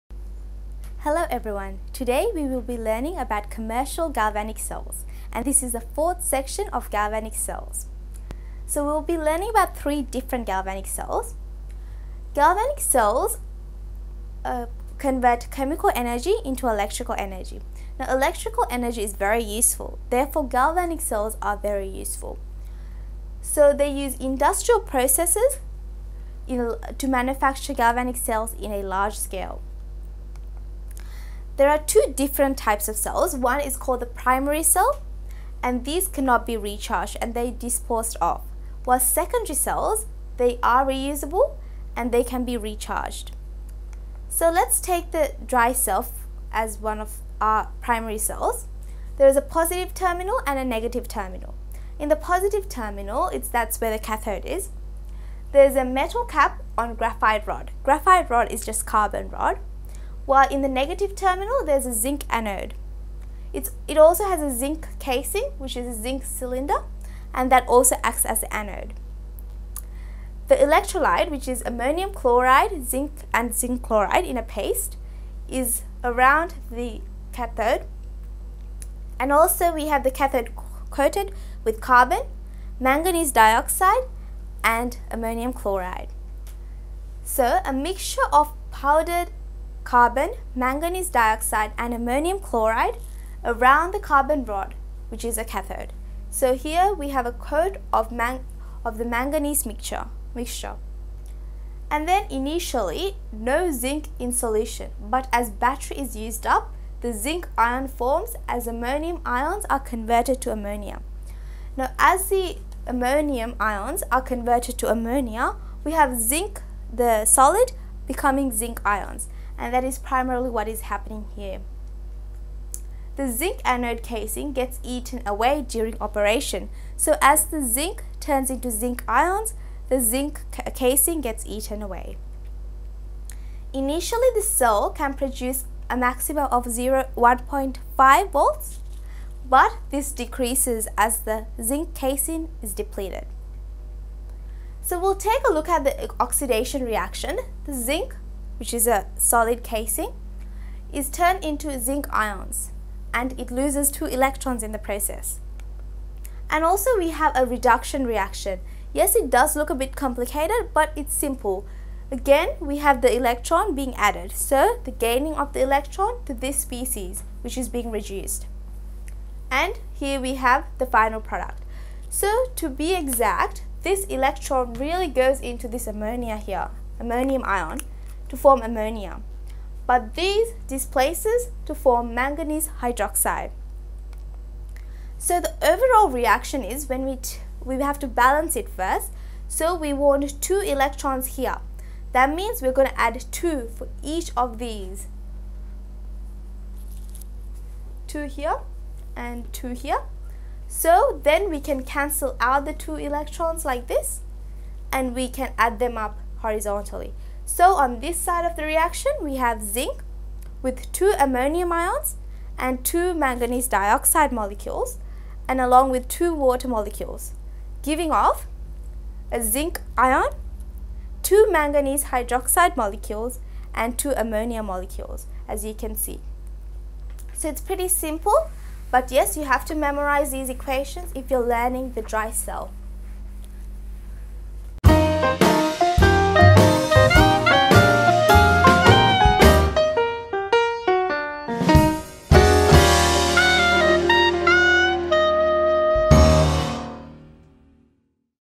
💯 Leclanché Cell | Commercial Galvanic Cells #1/4 | Electrochemistry
Since Galvanic cells convert chemical potential energy to electrical energy, they are very useful for commercial uses. Galvanic cells are manufactured on a large scale using industrial processes.

• Primary cells are disposable units that cannot be recharged.
• Secondary cells are re-usable units that can be recharged.

Keep in mind, this dot point only requires students to know 2 batteries: A dry cell/lead-acid cell and a button cell/ fuel cell/ vanadium redox cell/ lithium cell/ the Gratzel Cell.

Dry cell - Leclanche cell
• Electrolyte: NH4Cl, ZnCl2 paste
• Uses: Torches, radio, toys
• Advantages: Chemicals are a paste – easier to transport, small, lightweight, relatively cost-efficient, perform well at low temperatures
• Disadvantages: Short life, not rechargeable, bulkier than lithium cells, ammonia produced may expand and burst cell if used continuously